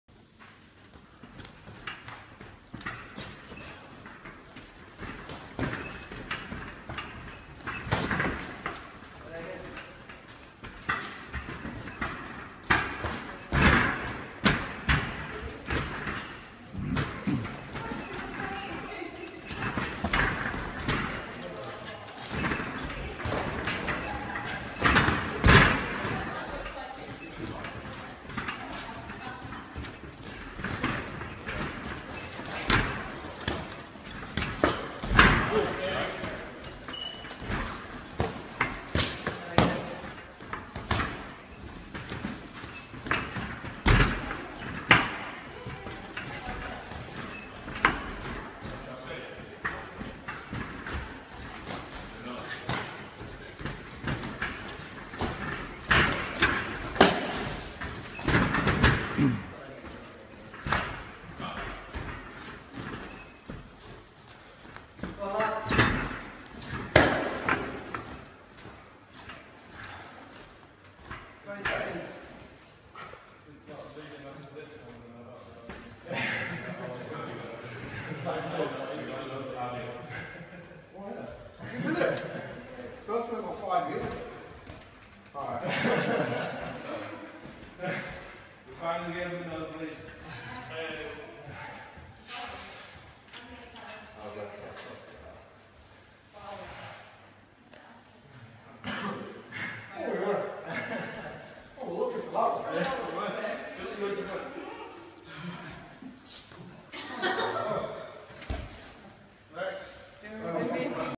A Playfoot vs J Clark Fight Club at Elemental Kickboxing Leeds
for 1-2-1 personal training